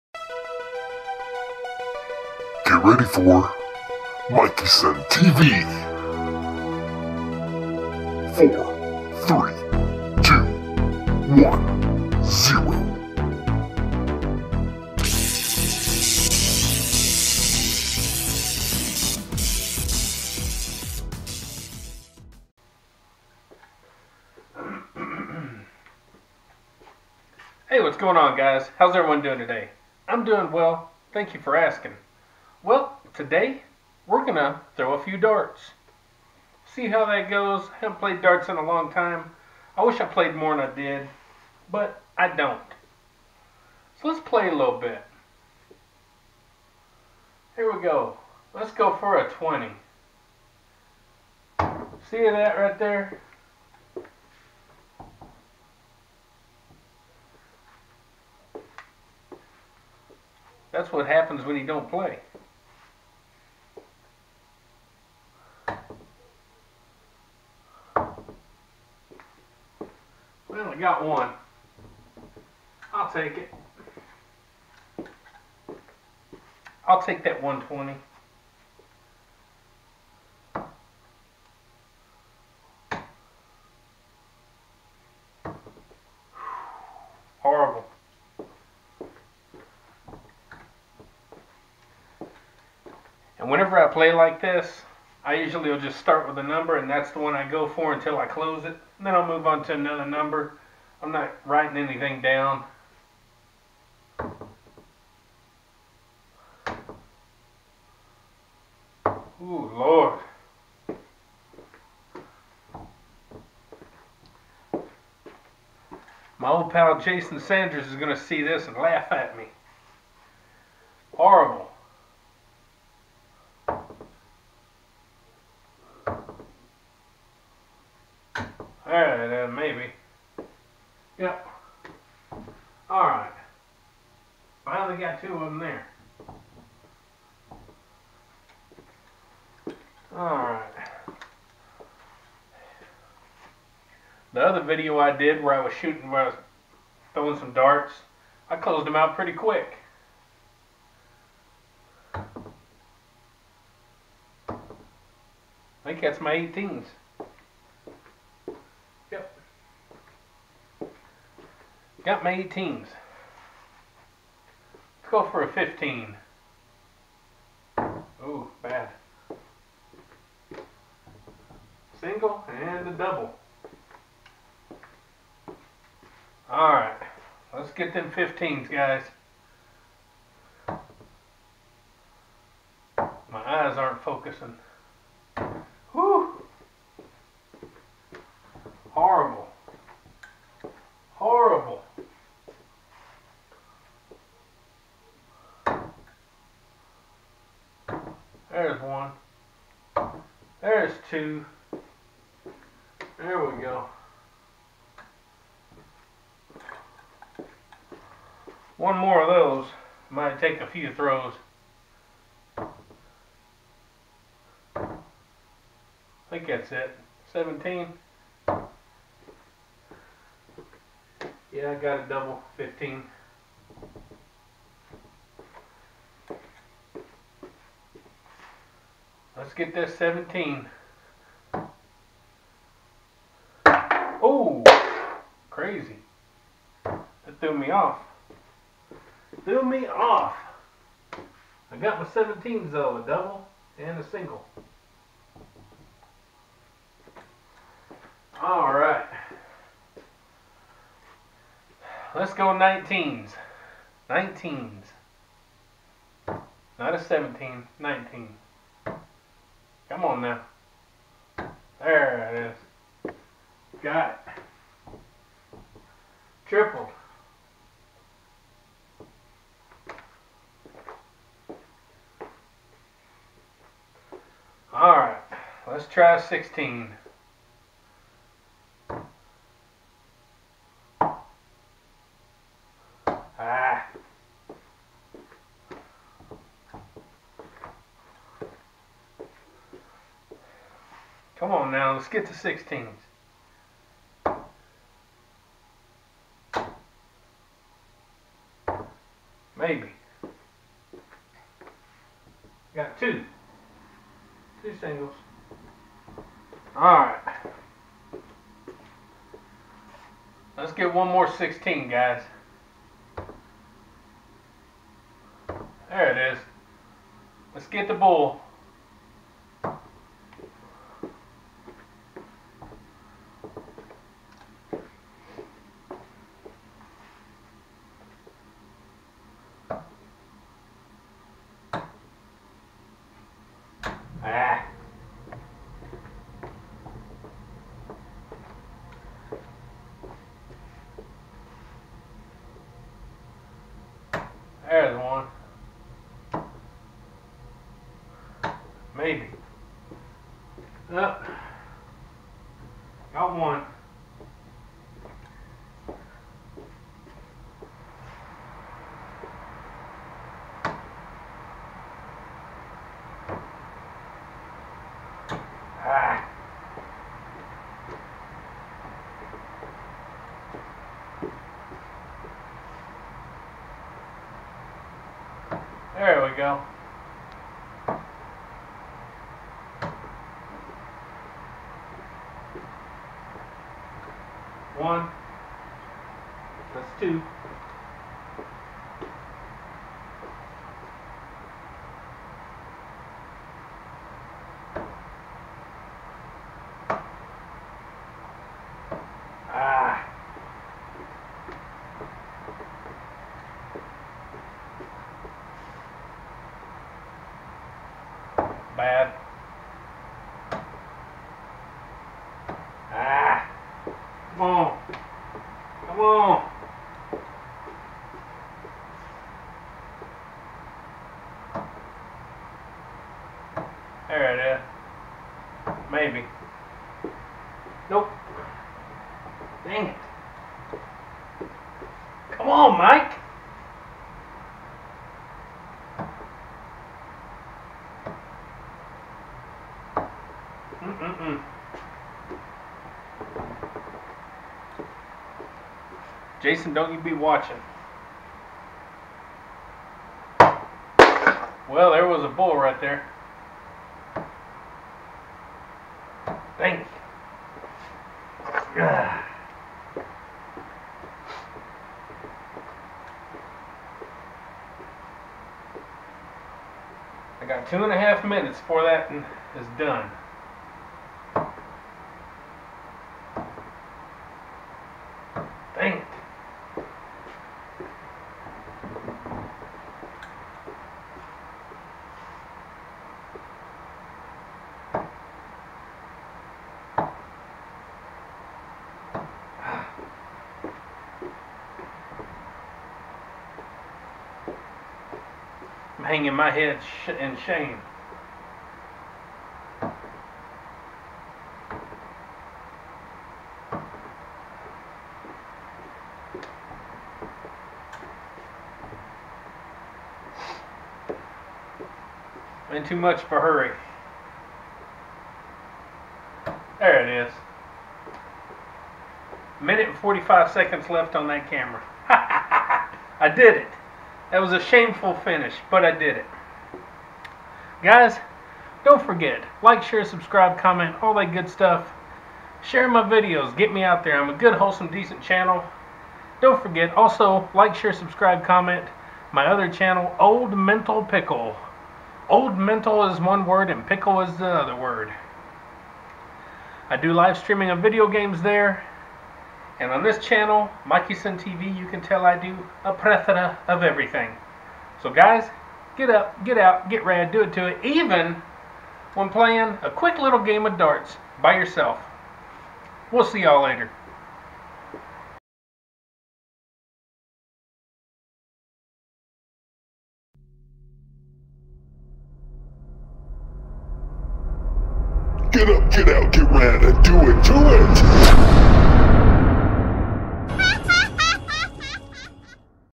Throwing Darts For Fun 2 20 21
I felt like throwing some darts if even for 10 minutes. I hope you enjoy the video and hope i inspire you to do something today..GET UP,GET OUT,GET RAD and DO IT TO IT!!